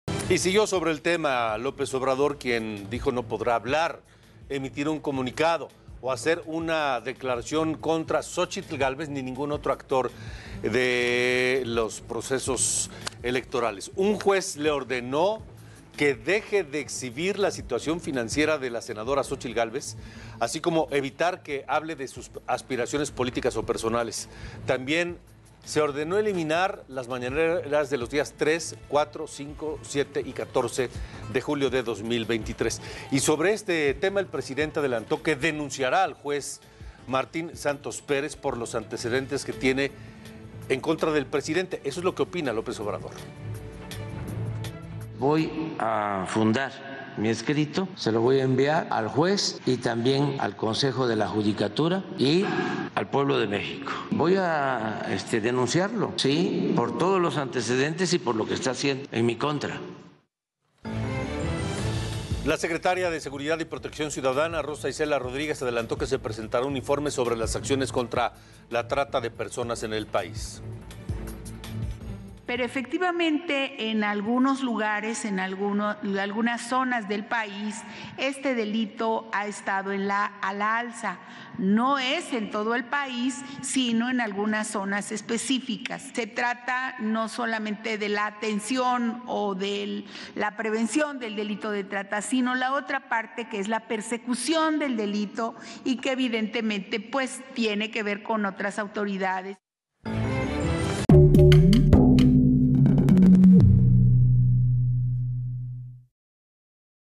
¿Berrinche o defensa de su libertad? AMLO quiere denunciar a juez que le impide hablar de X. Gálvez
El presidente Andrés Manuel López Obrador denunciará ante el Consejo de la Judicatura Federal (CJF) al juez Octavo de Distrito en Materia Administrativa, Martín Santos Pérez, quien concedió a la senadora panista Xóchitl Gálvez, una suspensión provisional que le ordena abstenerse de expresiones con “malicia efectiva”. Dijo que mediante un escrito detallará los antecedentes del juez y sus vínculos con personajes opositores a su gobierno.

“Ya estamos viendo cómo me quieren silenciar, un juez que habla, ¿cuál es la expresión que entrecomillaste? Con ‘’malicia efectiva’ le voy a contestar al juez por escrito porque la ‘malicia efectiva’ es de su parte. Y le voy a enviar también formalmente un escrito al Consejo de la Judicatura en contra del juez por todos sus antecedentes y por su actuación sectaria, ilegal e injusta, además politiquera”, argumentó.